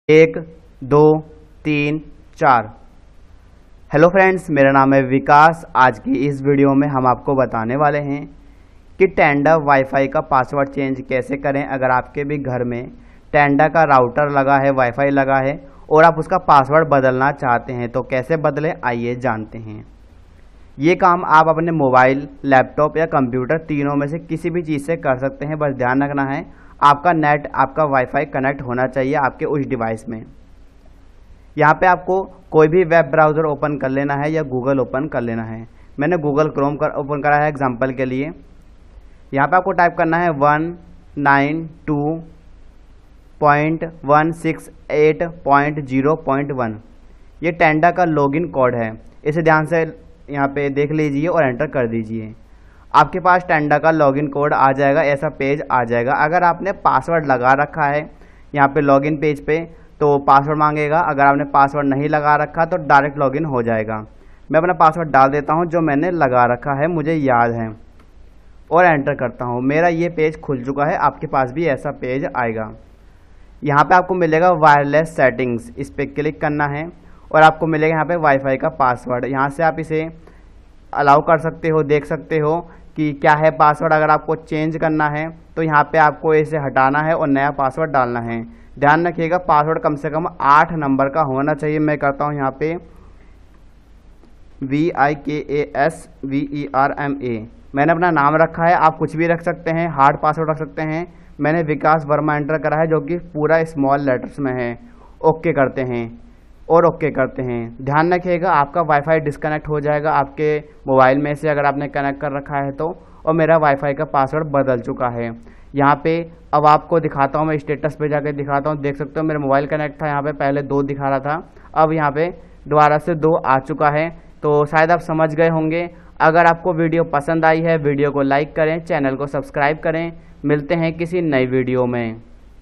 Tenda Wifi Password Change | Wifi Ka Password Kaise Change Kare Tenda
Aaj Ki Is Video Me Hum Aapko Batane Wale Hai Ki Tenda Wifi Ka Password Kaise Change Karte Hai To Aap Aaj Ka Yeah Video Poora Jarur Dekhe Or Like Karna Naa Bhoole

Content Covered :-
Wifi Password Change Tenda Router
Tenda Ka Password Kaise Change Kare